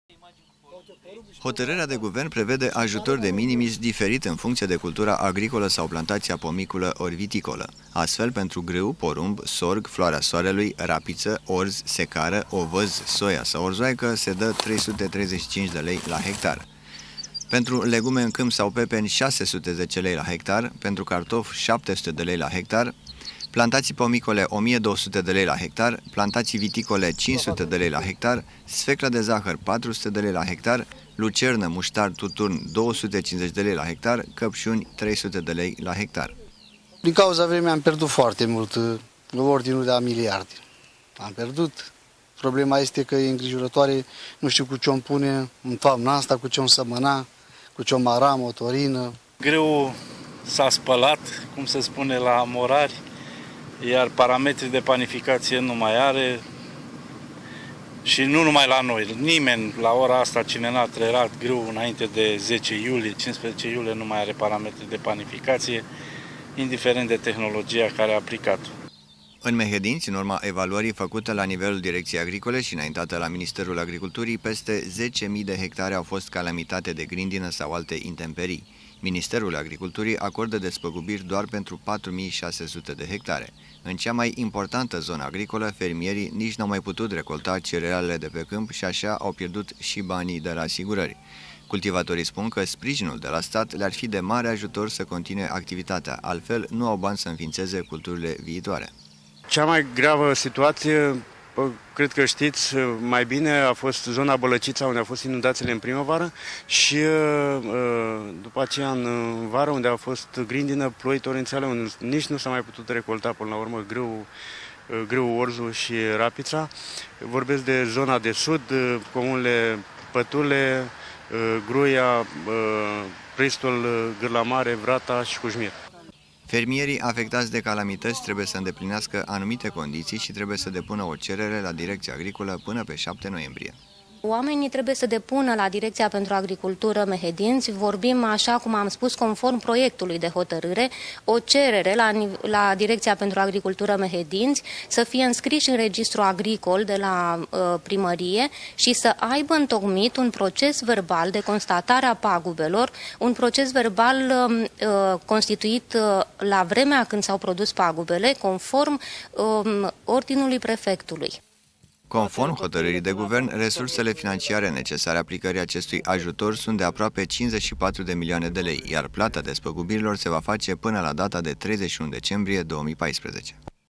SPRIJIN PENTRU FERMIERII CALAMITĂŢI
Guvernul Ponta vrea să acorde despăgubiri fermierilor ale căror culturi agricole au fost afectate de vremea rea din acest an. Este vorba la o primă evaluare de peste 140.000 de hectare calamitate din cauza condiţilor meteorologice severe, la nivelul întregii ţări. Hotărârea stabileşte şi sumele pe care fermierii din sectorul vegetal le vor primi pentru fiecare hectar afectat, dar limita maximă a sumei pe care o poate primi o exploataţie agricolă nu poate fi mai mare de 15 mii de euro.